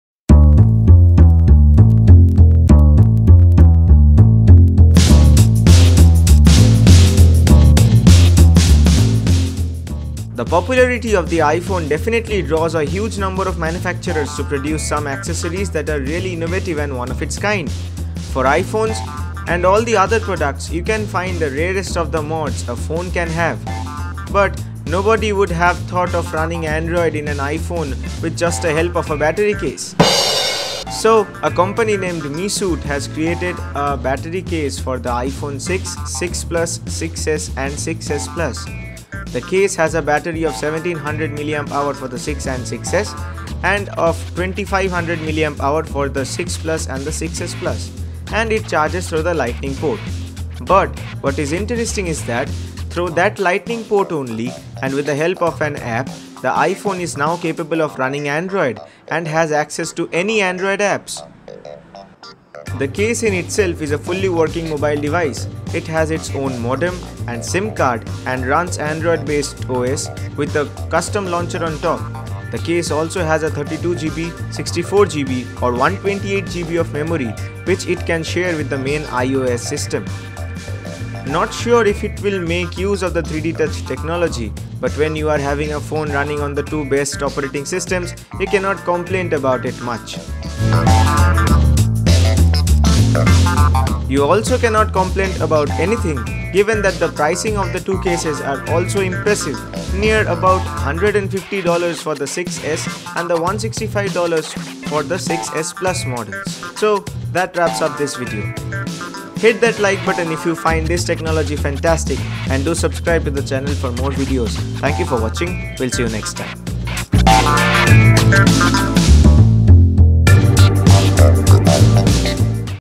iPhone Case That runs Android !!
Mesuit is a company that has made a battery case for the iPhone 6/6plus/6s/6splus with a rather different speciality.
The case has it's own sim cards, and modem, and runs android !!